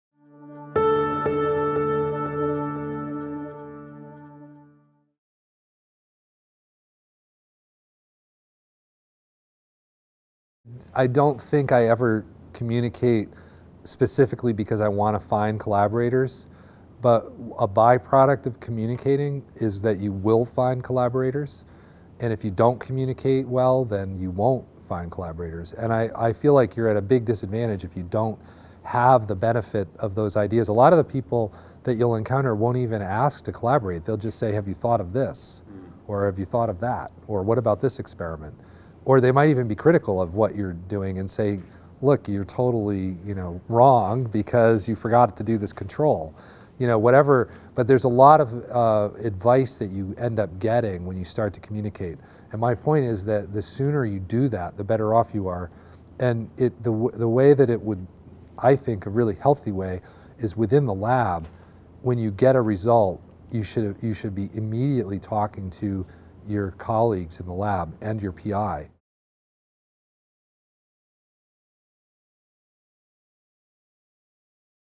"A byproduct of communicating is that you will find collaborators" Craig Mello, Nobel Laureate
Nobel Laureate Craig Mello answers the question 'Have your communication skills helped you find collaborators?'

Craig Mello, who received the Nobel Prize for his work on RNAi, took part in a Nobel Prize Inspiration Initiative event.

Through the Initiative, Nobel Laureates give career advice for young scientists, explain their discoveries and give insights into life after the Nobel Prize.